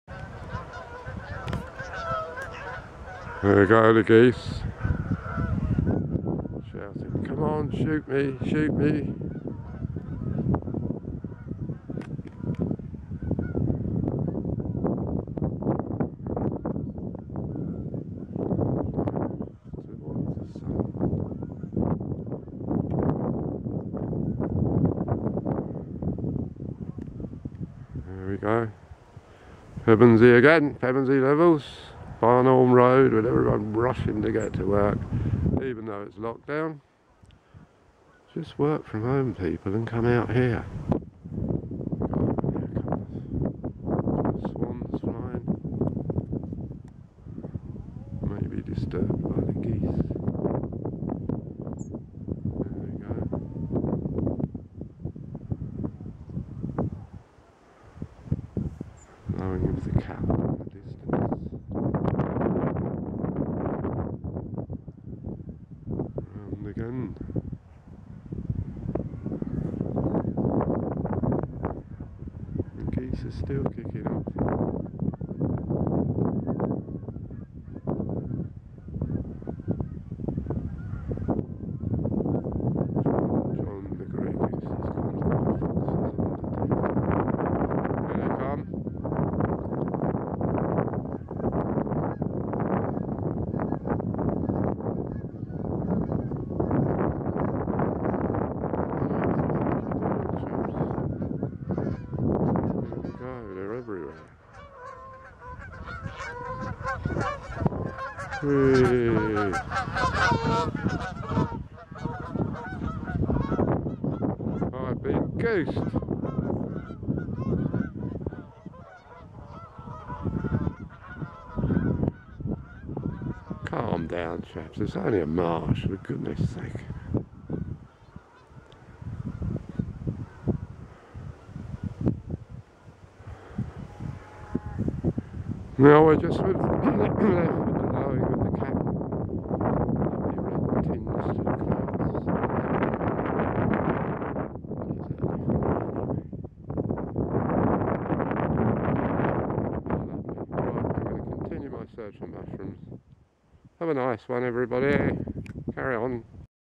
GOOSED ON PEVENSEY LEVELS.
Early morning on the levels and the geese are flying in.